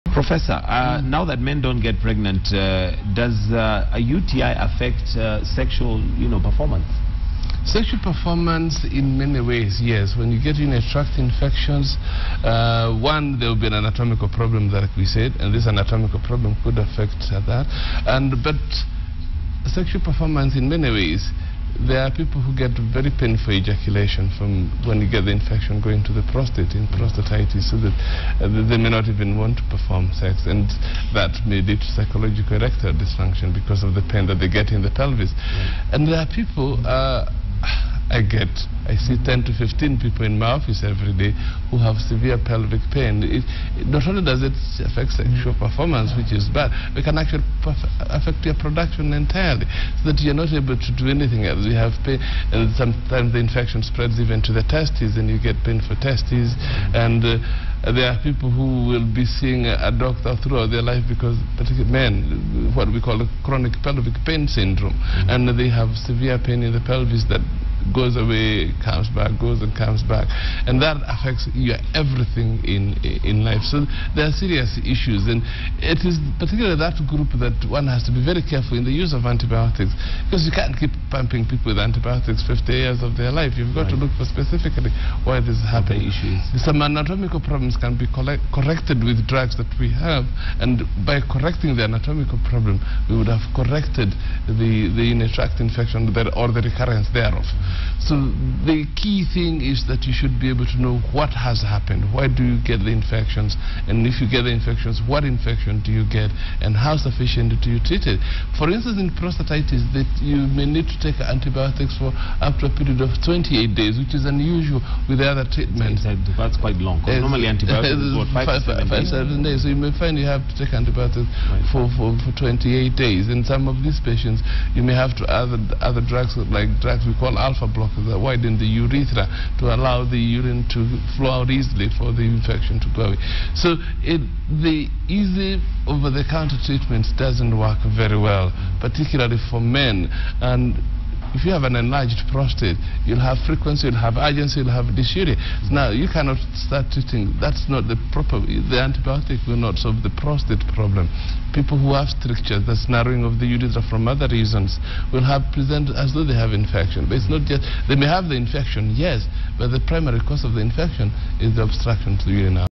Urologist Dr. Mungai explains how Urinary tract infection affects sex performance in men
Urologist Dr. Mungai explains how Urinary tract infection affects sex performance in menWatch KTN Live